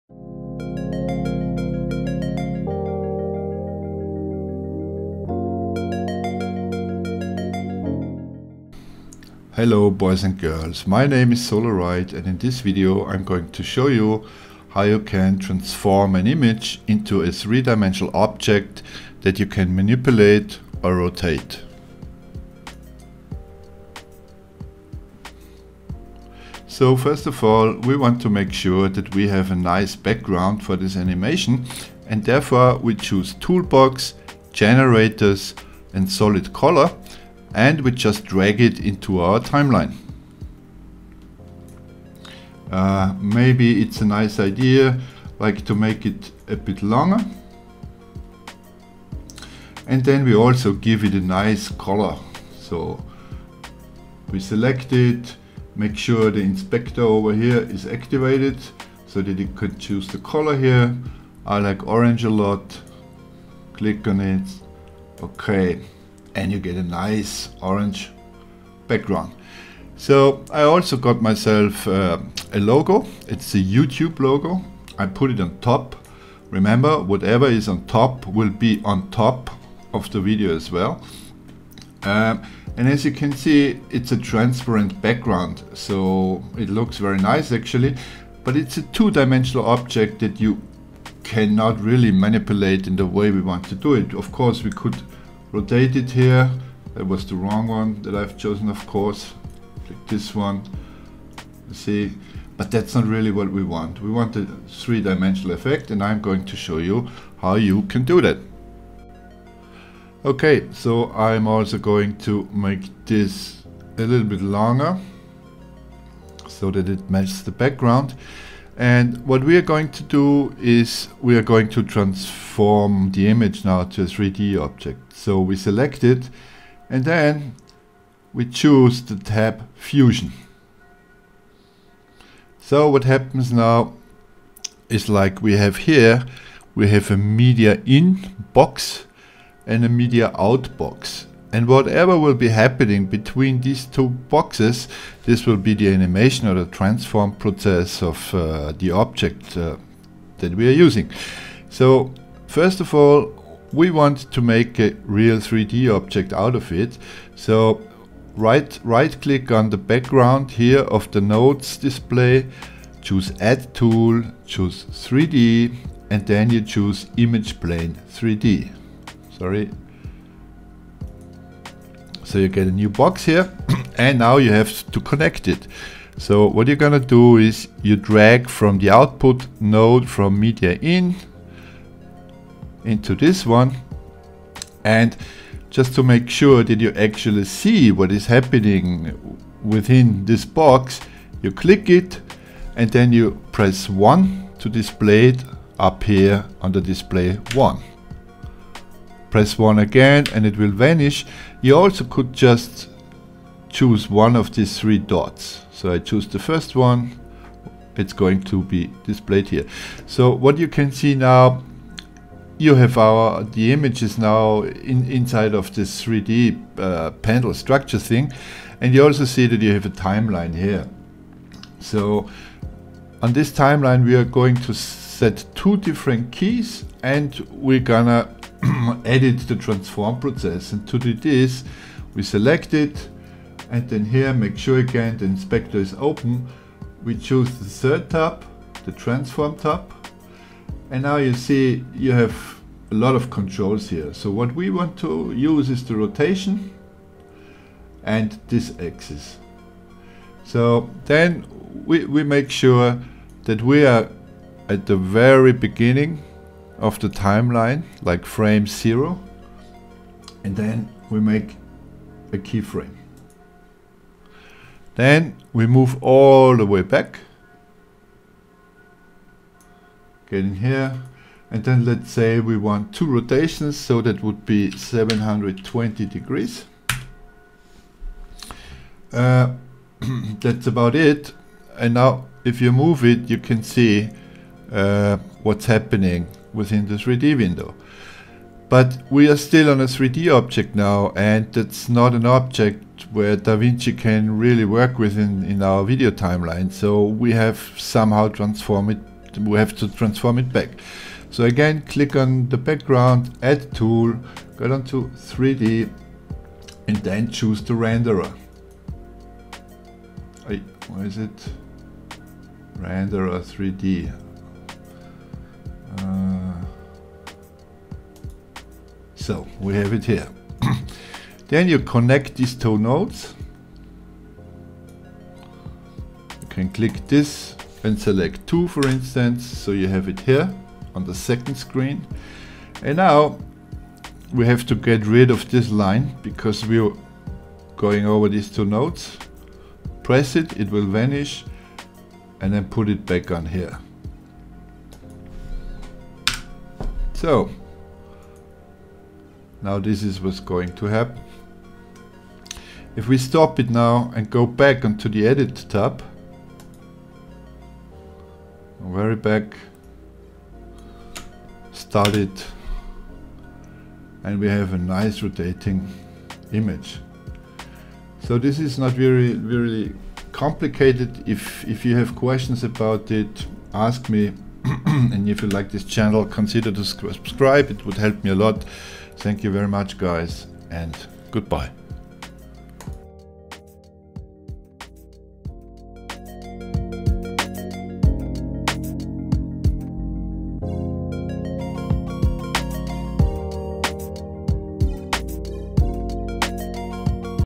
Davinci Resolve 16 - How To make an Image 3D and rotate it
In this Davinci Resolve 16 Tutorial we are going to transform an image into a 3D object, that you can manipulate. We will use fusion to do that.